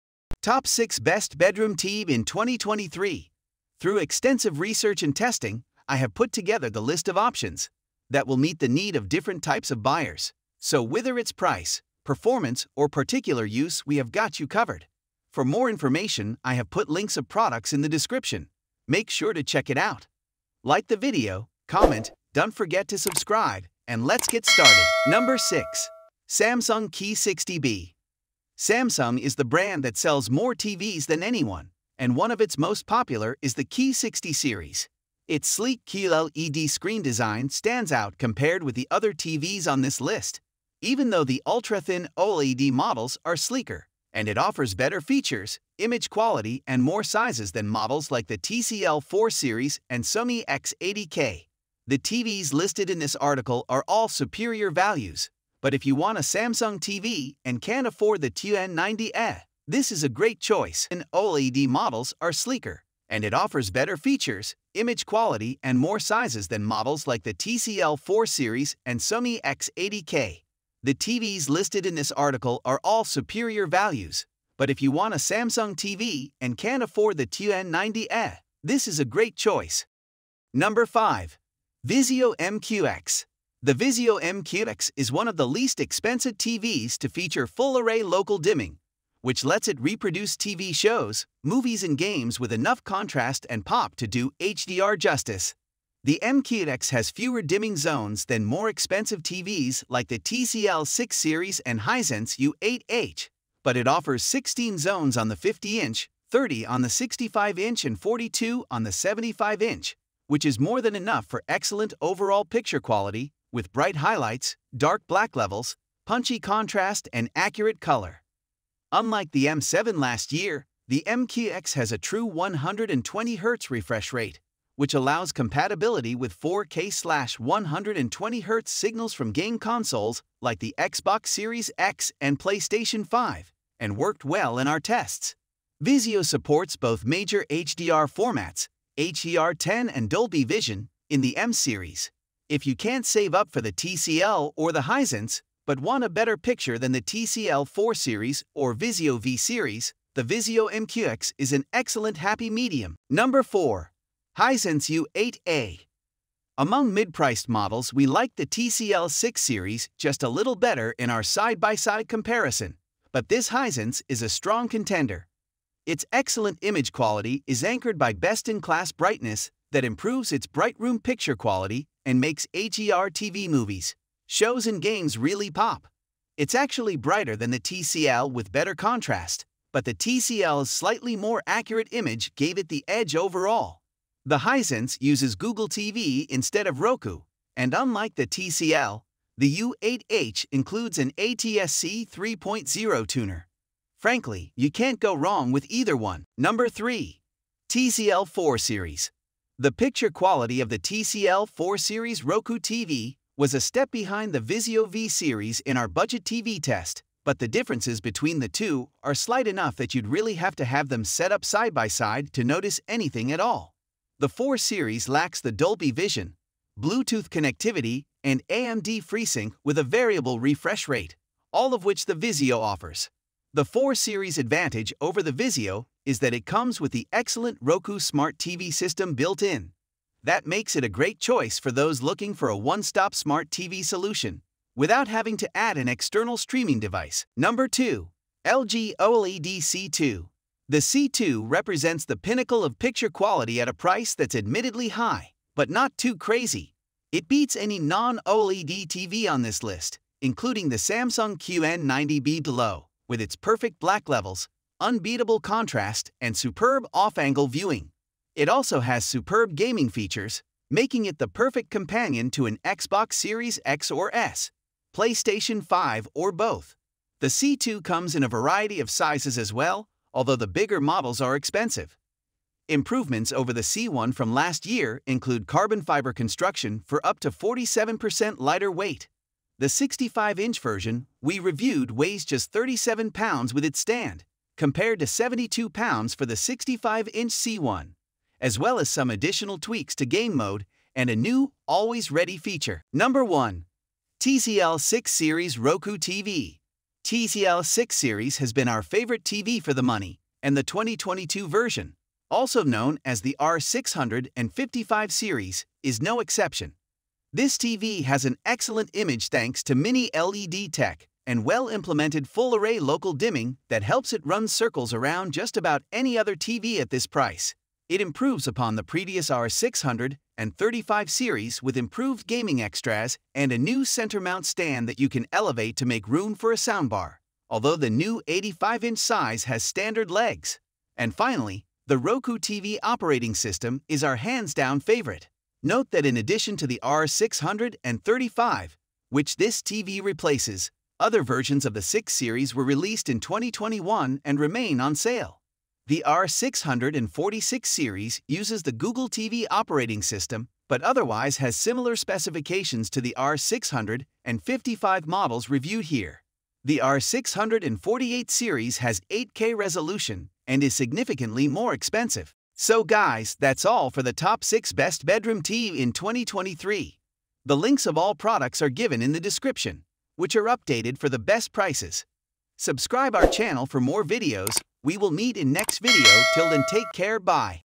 Top 6 Best Bedroom TV in 2024
➜ Links to the 6 Best Bedroom TV 2023 we listed in this video:

►Links►

➜ 6- SAMSUNG Q60B
➜ 5- VIZIO MQX
➜ 4- HISENSE U8H
➜ 3- TCL 4-SERIES
➜ 2- LG OLED C2
➜ 1- TCL 6-SERIES ROKU